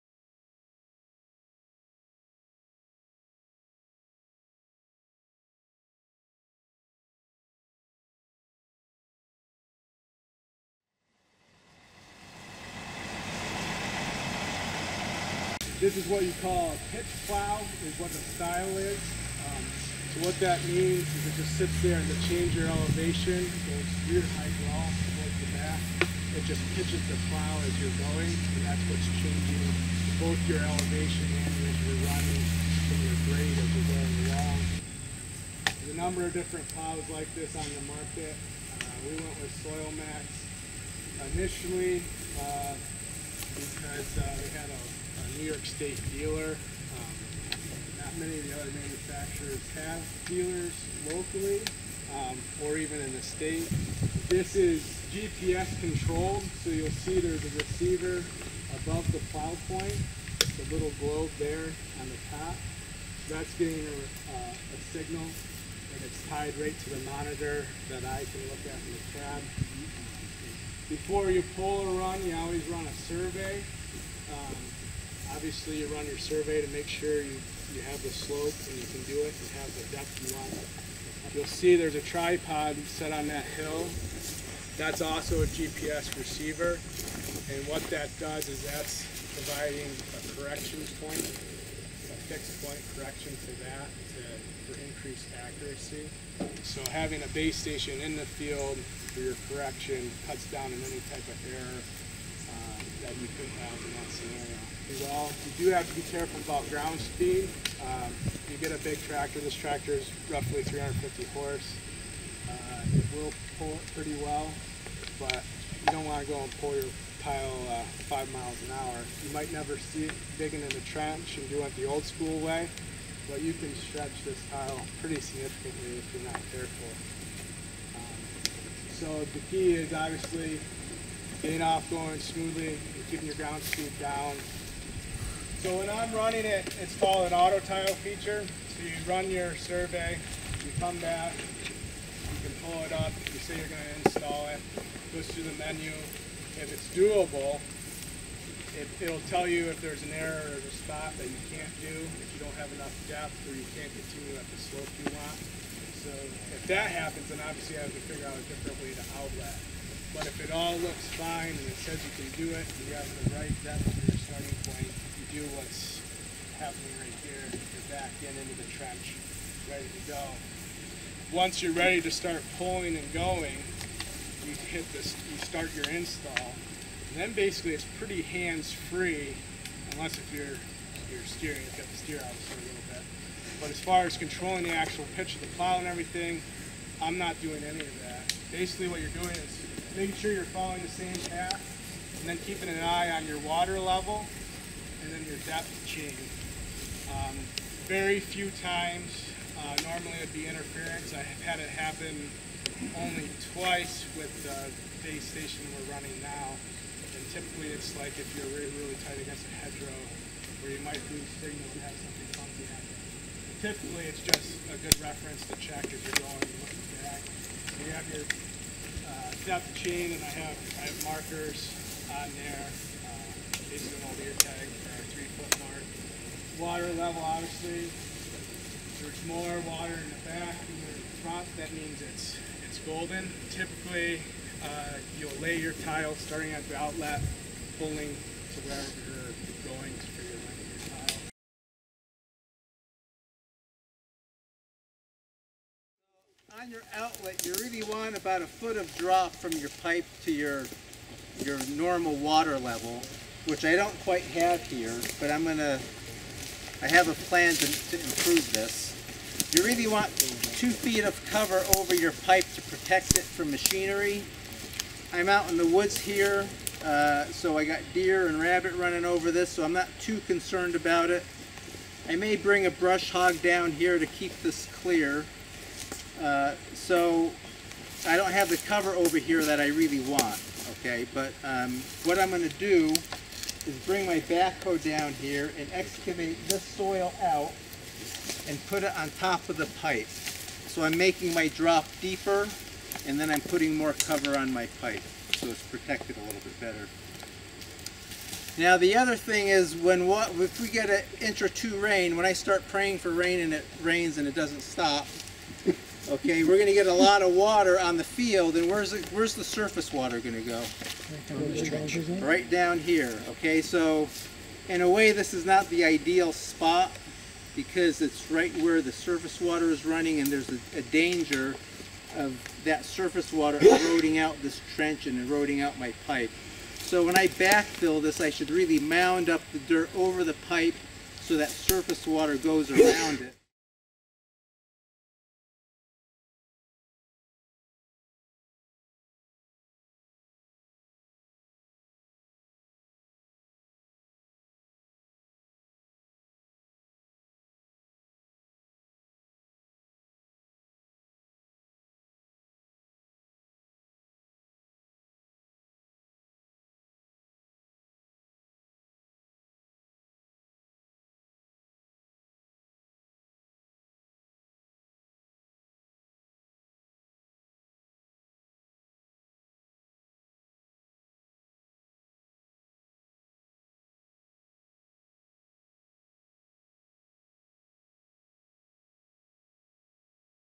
Tile Drainage Field Day 9-1-15
Highlights from the Tile Drainage Field Day presented by Aaron Gabriel, Agronomist, Cornell Cooperative Extension Capital Area Agriculture and Horticulture Program.